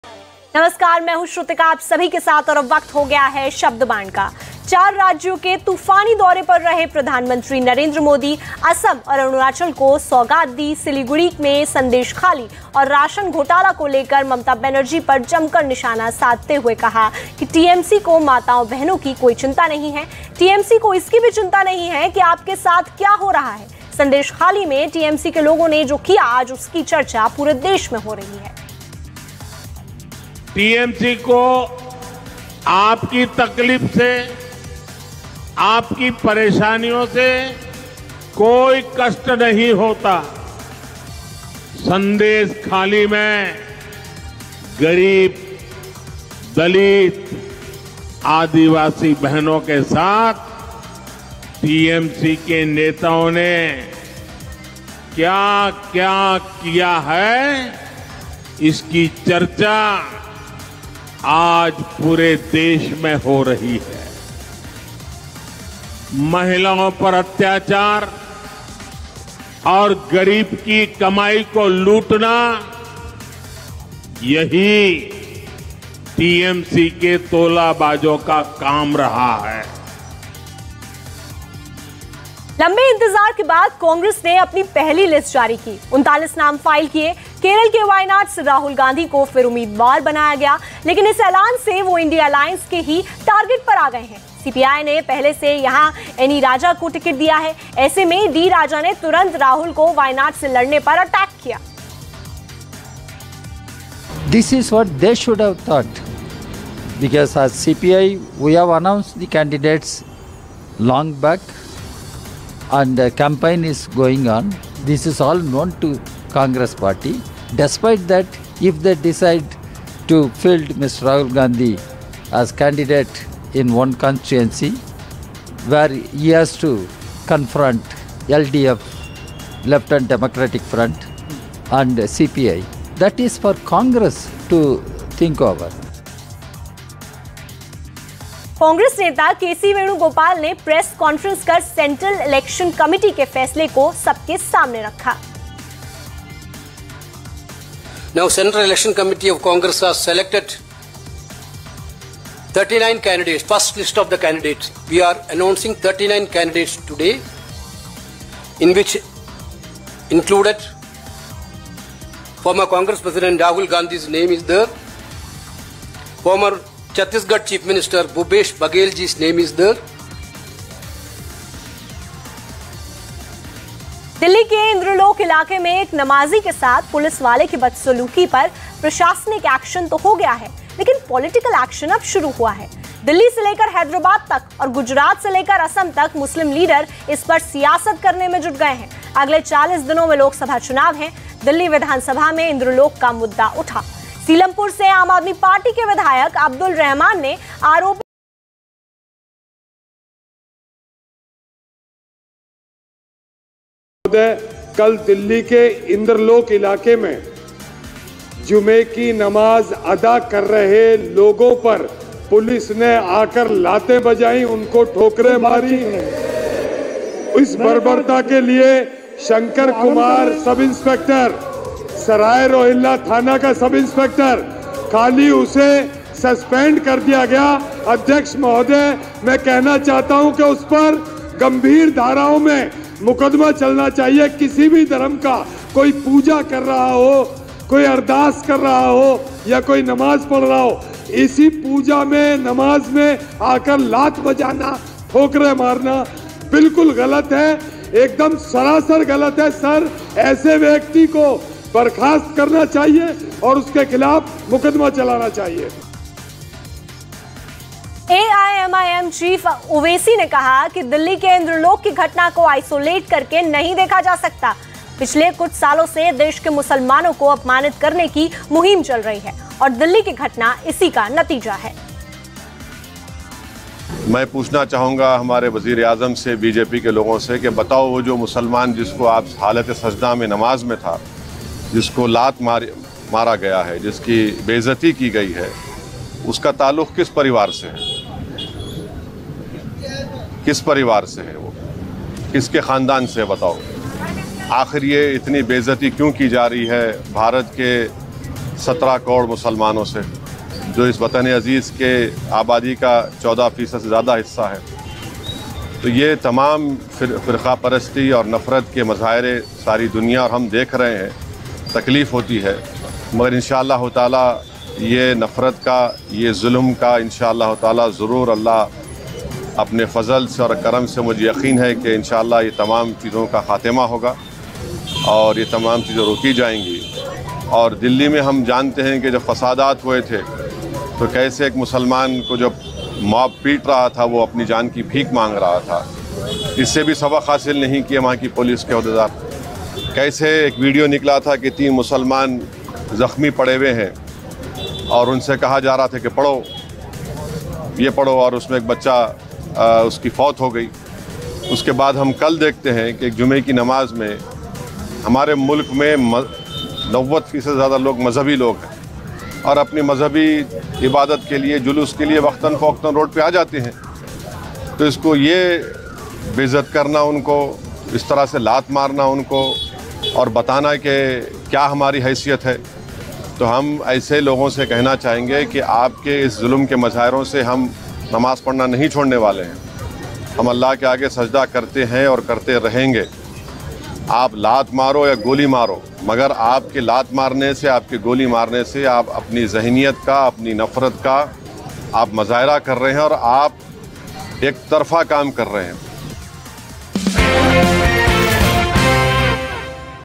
Sandeshkhali Case: 'TMC को माताओं और बहनों की कोई चिंता नहीं', TMC पर भड़के PM Modi | Speed News
Sandeshkhali Case: प्रधानमंत्री नरेंद्र मोदी ने भ्रष्टाचार और संदेशखाली और वंशवाद की राजनीति के पीड़ितों मुद्दे पर तृणमूल कांग्रेस (टीएमसी) की आलोचना की।